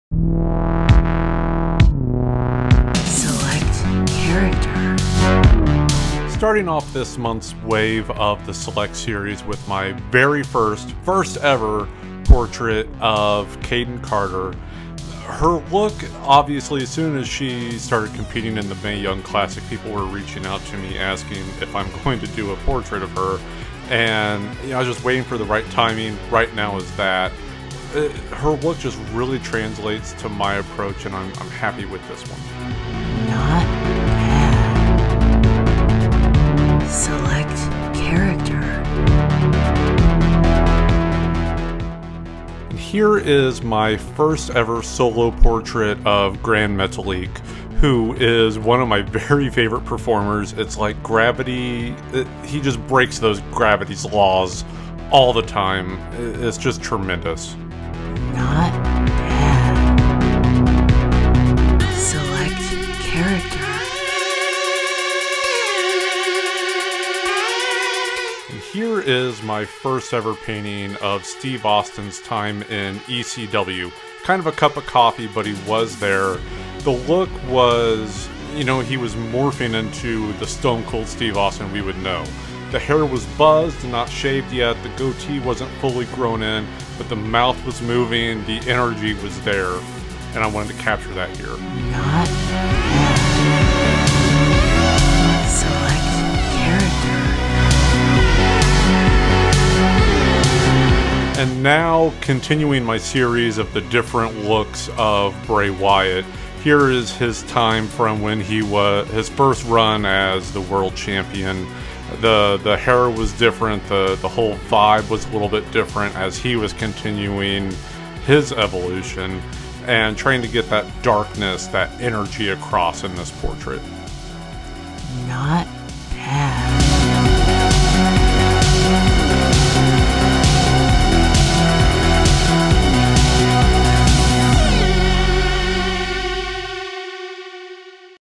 Select Series – Kayden Carter, Gran Metalik, Steve Austin, and Bray Wyatt: WWE Canvas 2 Canvas
Artist Rob Schamberger brings more new portraits to his on-going video game-inspired Select Series with this edition featuring up-and-coming NXT Superstar Kayden Carter, The Crown Jewel of Lucha Libre, “Stone Cold” during his short-tenure in ECW, and “The Eater of Worlds” during his first run as WWE World Champion!
Follow WWE on YouTube for more exciting action!